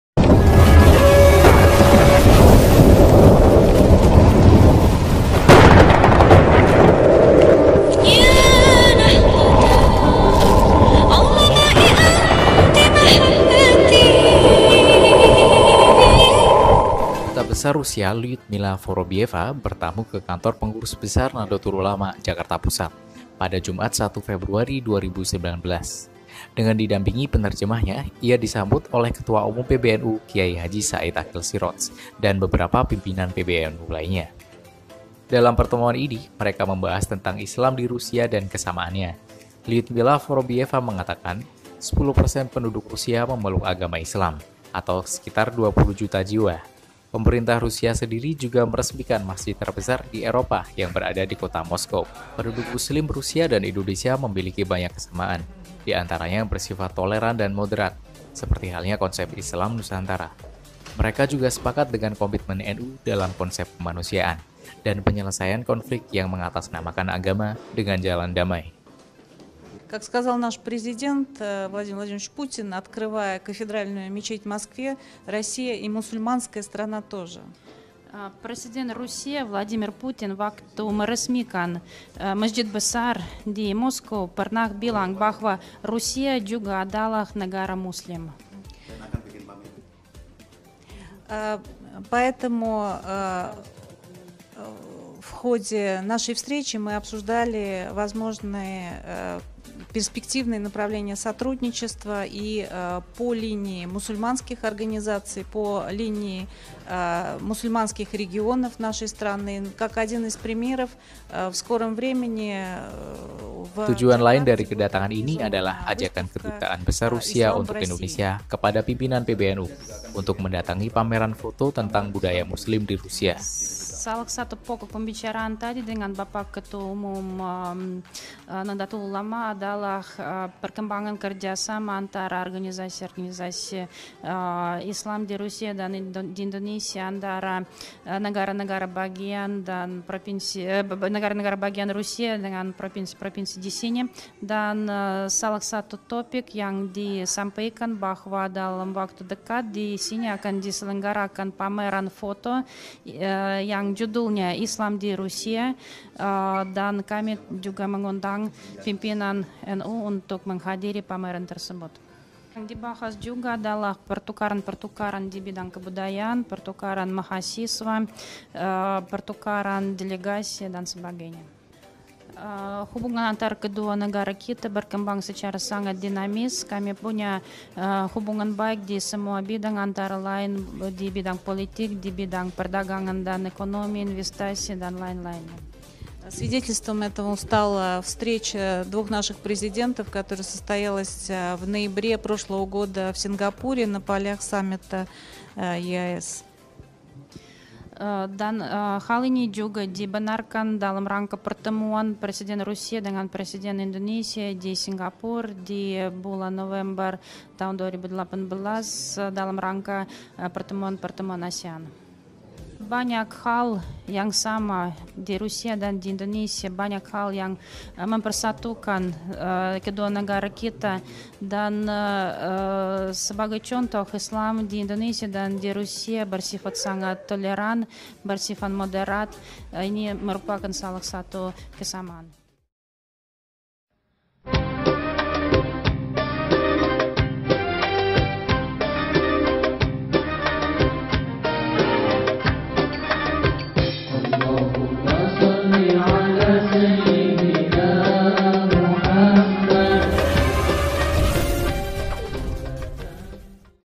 Dubes Rusia Kunjungi PBNU
Perkembangan islam di Rusia